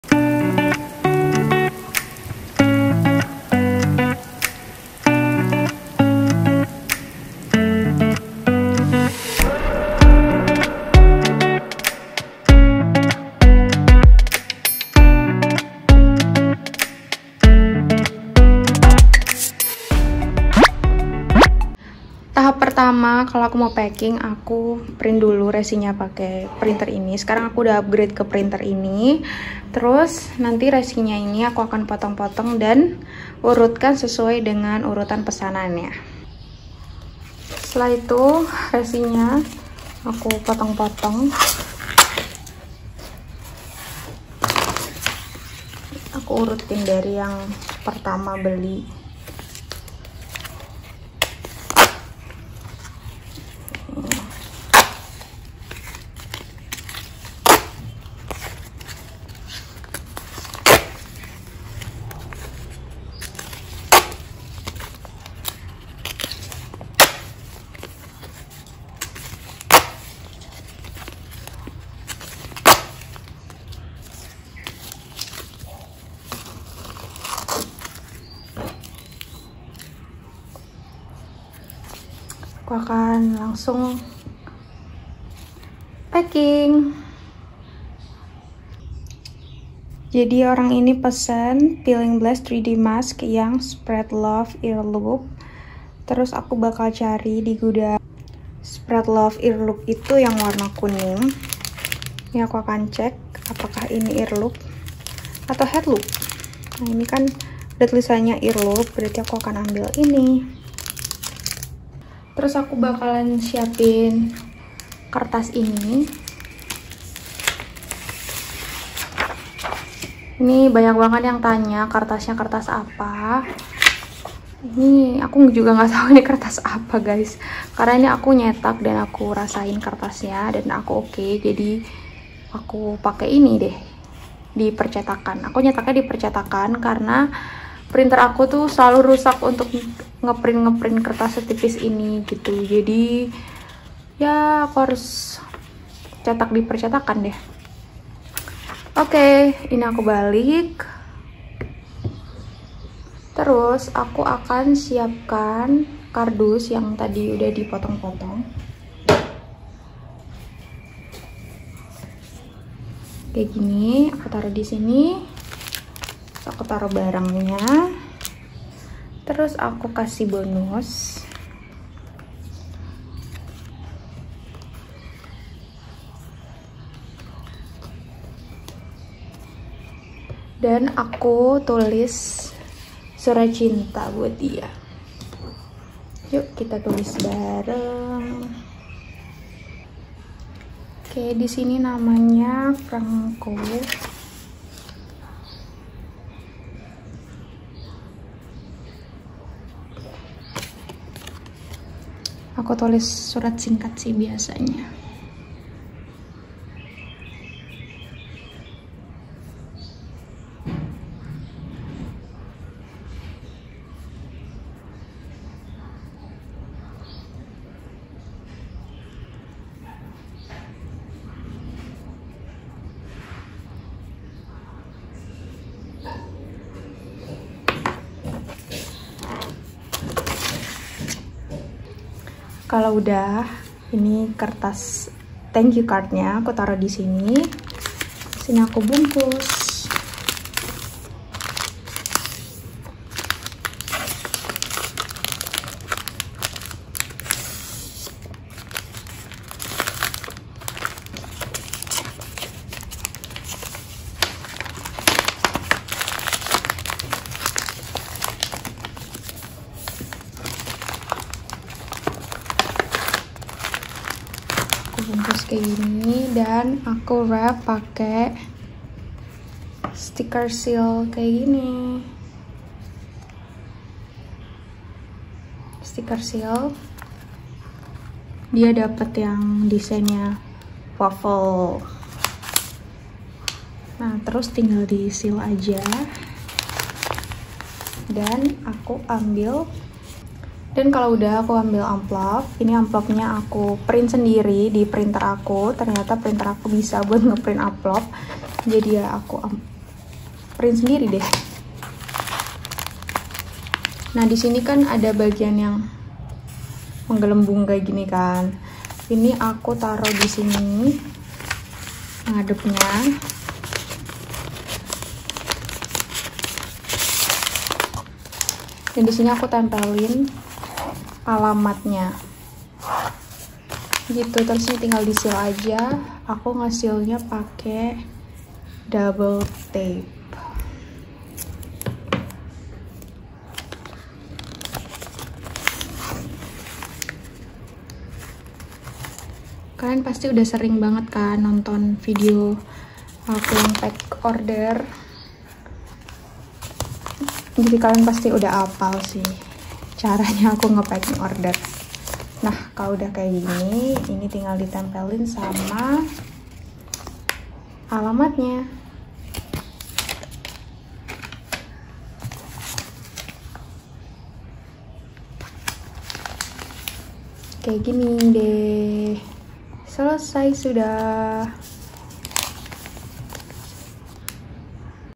How I Pack My Orders + Short Explanation | Indonesia
FIND ME

Camera: iPhone 11
Editing: Vllo, Procreate (for thumbnail)